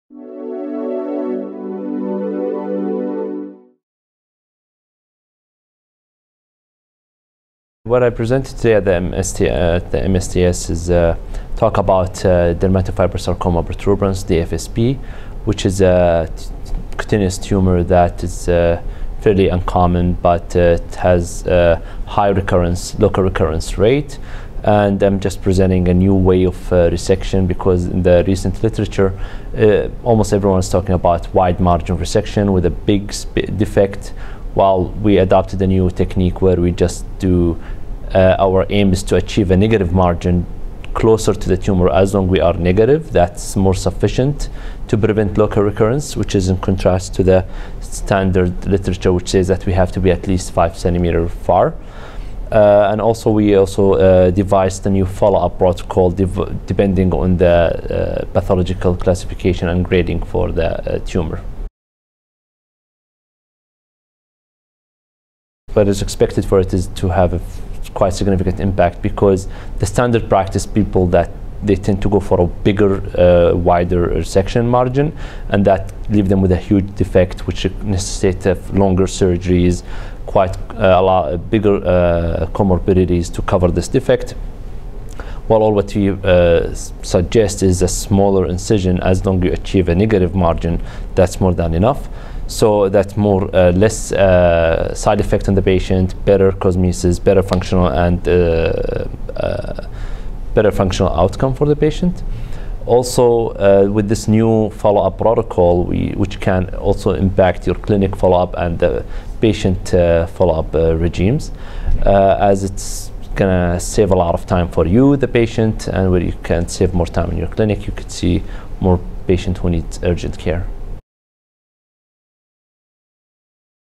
Dr. Alshaygy on Resection Margin for Dermatofibrosarcoma Protuberans
Ibrahim S. Alshaygy, MD, MSC, a clinical fellow in Orthopedic Oncology at Mount Sinai Hospital, discusses research suggesting a smaller surgical resection margin for dermatofibrosarcoma protuberans (DFSP).